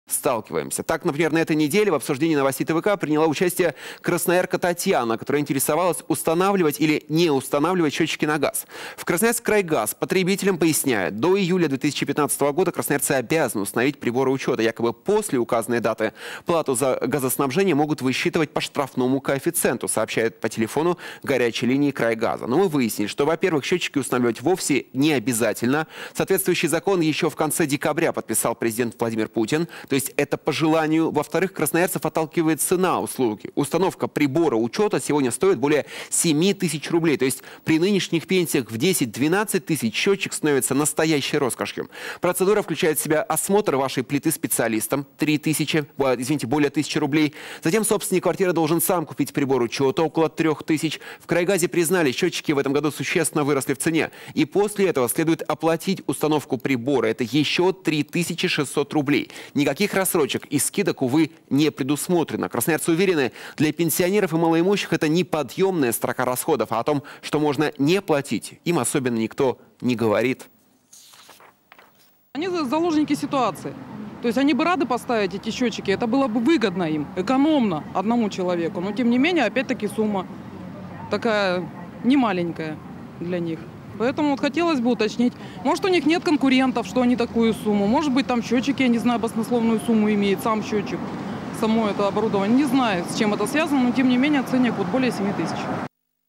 Необязательные "обязательные счетчики" на газ.
На этой неделе в Обсуждении Новостей ТВК приняла участие красноярка Татьяна, которая интересовалась, устанавливать или не устанавливать счётчики на газ? В "КрасноярскКрайГазе" потребителям поясняют, что до июля 2015-го года красноярцы обязаны установить приборы учёта. Якобы после указанной даты плату за газоснабжение могут высчитывать по штрафному коэфициенту, сообщают по телефону горячей линии "КрайГаза". Но мы выяснили, что, во-первых, счётчики устанавливать вовсе не обязательно! Соответствующий закон еще в конце декабря подписал президент Путин. То есть это по желанию. Во-вторых, красноярцев отталкивает цена услуги. Установка прибора учёта сегодня стоит более семи тысяч рублей. То есть при нынешних пенсиях в 10-12 тысяч счётчик становится настоящей роскошью. Процедура включает в себя осмотр вашей плиты специалистом - более тысячи рублей. Затем собственник квартиры должен сам купить прибор учета - около трёх тысяч . В "Крайгазе" признались - счётчики в этом году существенно выросли в цене. И после этого - следует оплатить установку прибора - ещё три тысячи шестьсот рублей. Никаких рассрочек и скидок, увы, не предусмотрено. Красноярцы уверены - для пенсионеров и малоимущих это неподъёмная строка расходов. А о том, что можно не платить - им особенно не говорят.